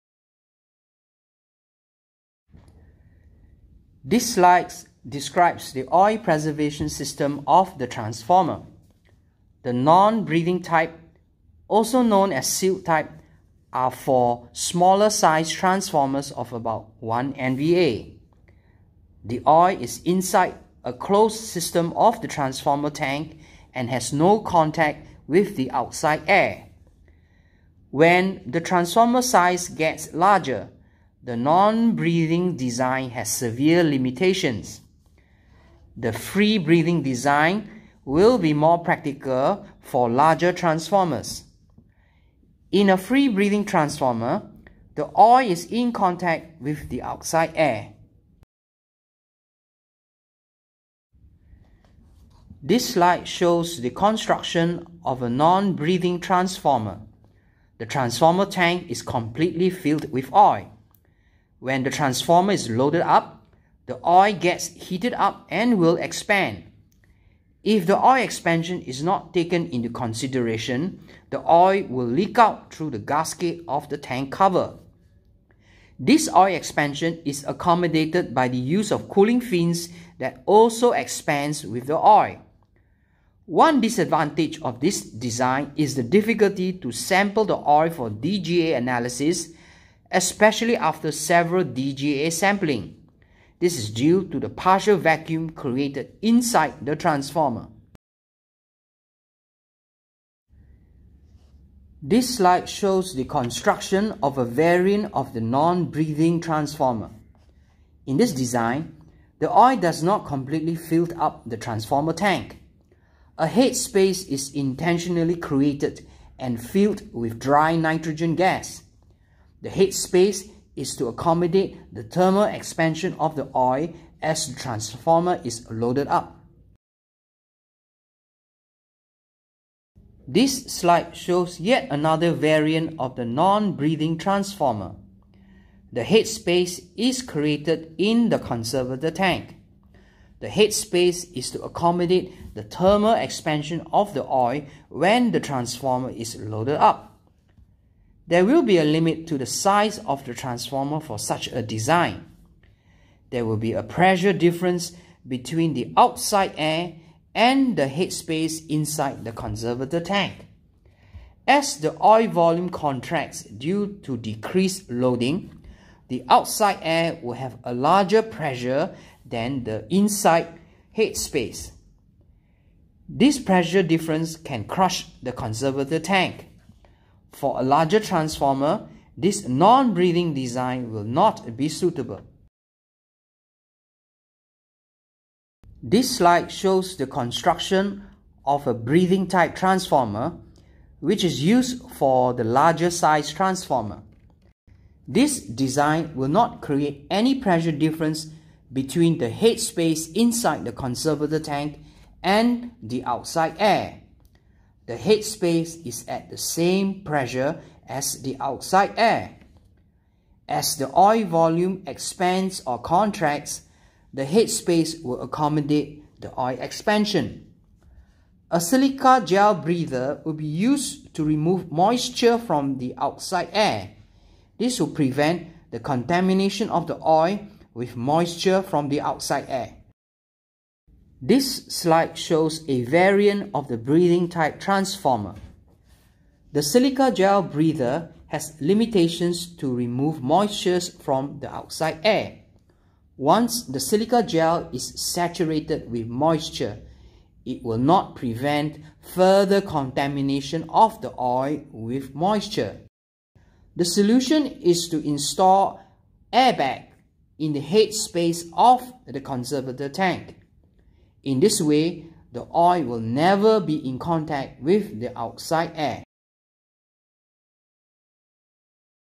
Part 1 - The construction of Oil Transformers
Dissolved Gas Analysis Of Transformer Oil

Presented by Er. Lee Wai Meng (Managing Director of ON Engineers Pte Ltd), on July  2020